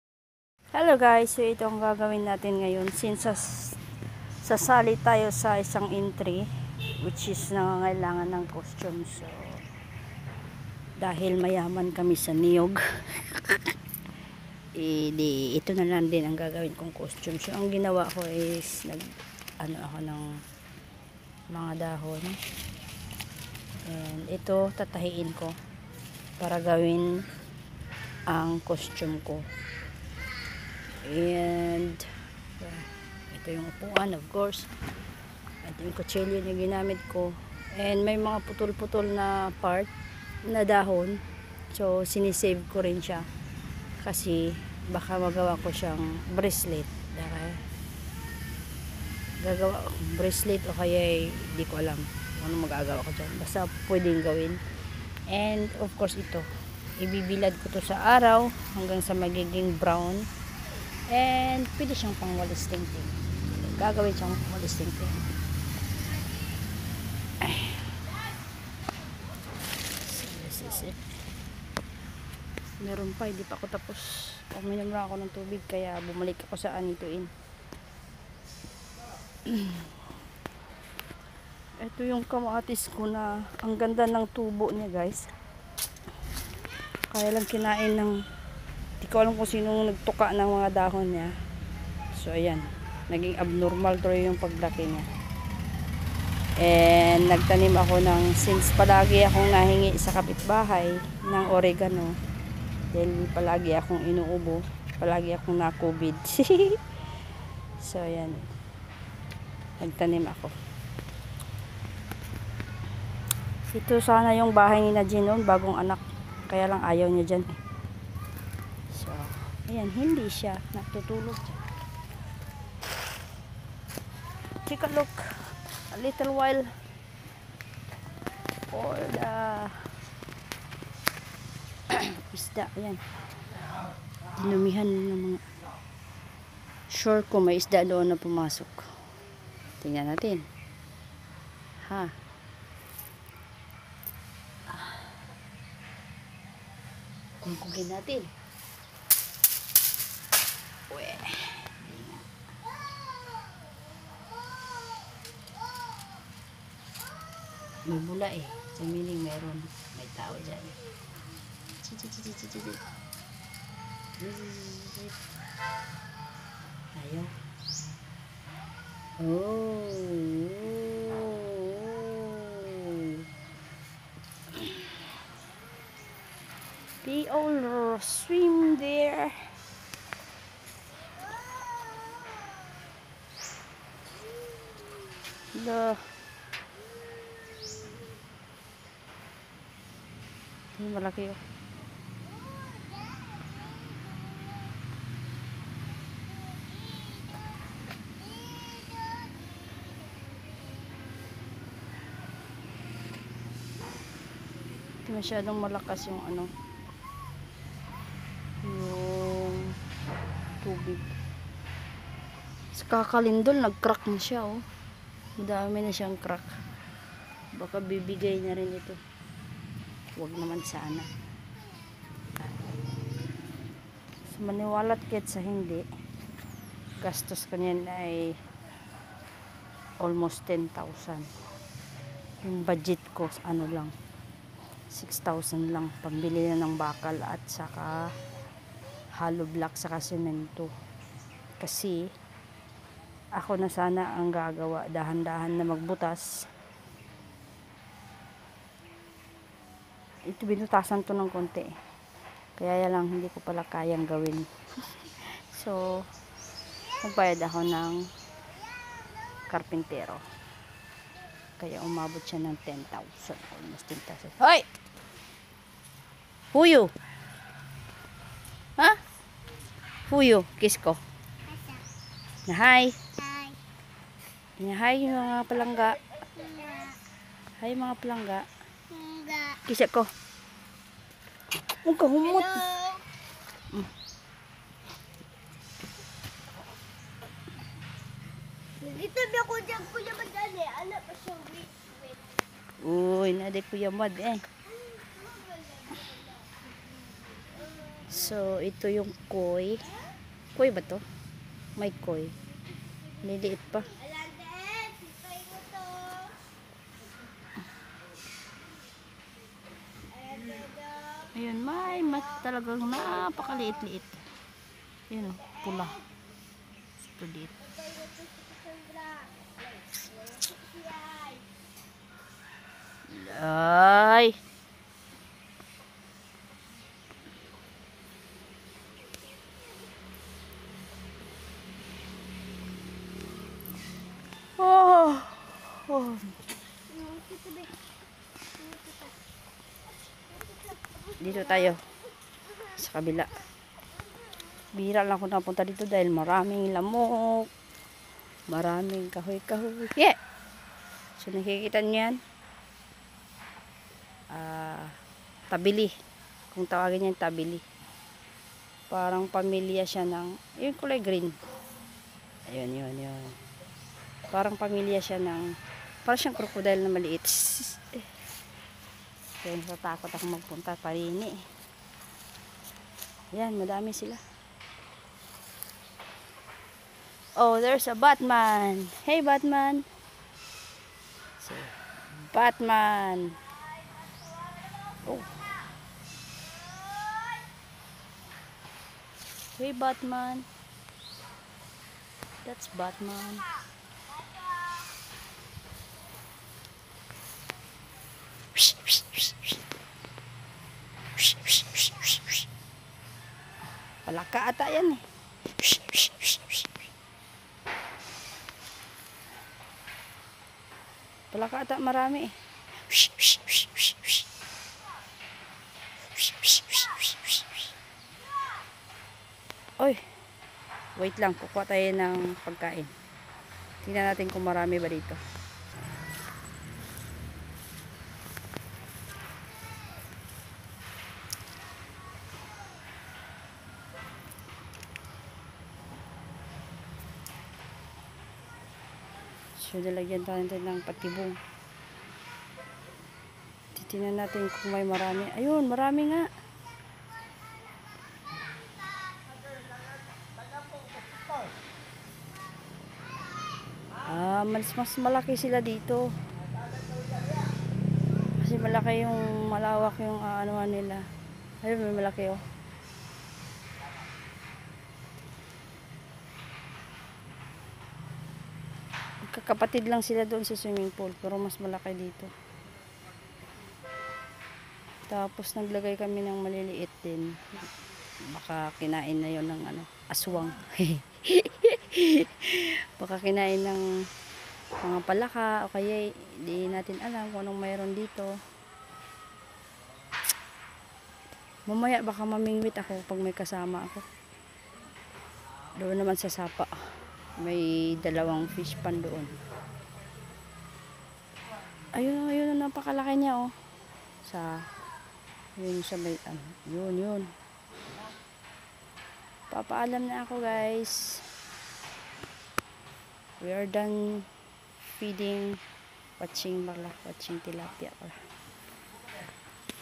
HOW TO MAKE SKIRT FOR HAWAII DANCE FROM COCONUT LEAF (Anything under the sun) Part II
how to make walis tingting..main purpose is making skirt out of coconut leaves